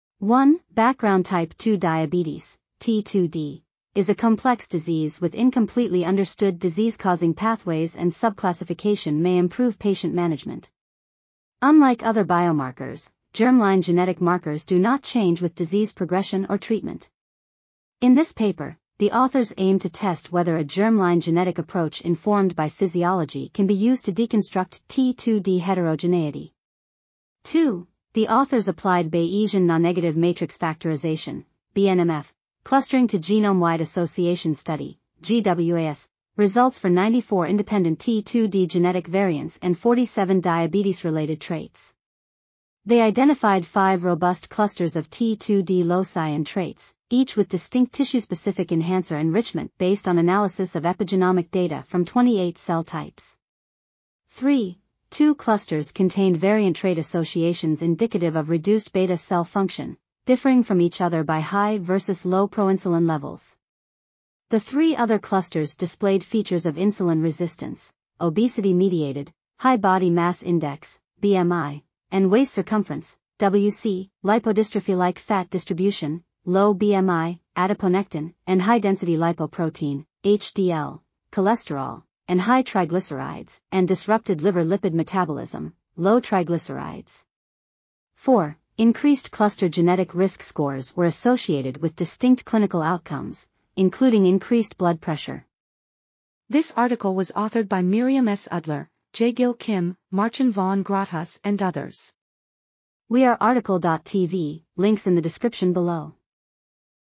Type 2 diabetes genetic loci informed by multi-trait associations point to disease me... | RTCL.TV
### Keywords ###

### Article Attribution ###
Title: Type 2 diabetes genetic loci informed by multi-trait associations point to disease mechanisms and subtypes: A soft clustering analysis.
Authors: Miriam S Udler, Jaegil Kim, Marcin von Grotthuss, Sílvia Bonàs-Guarch, Joanne B Cole, Joshua Chiou, Christopher D. Anderson on behalf of METASTROKE and the ISGC, Michael Boehnke, Markku Laakso, Gil Atzmon, Benjamin Glaser, Josep M Mercader, Kyle Gaulton, Jason Flannick, Gad Getz ,and Jose C Florez

### Video Timestamps ###
0:00:00 - Summary
0:01:36 - Title
0:01:42 - Tail
0:01:47 - End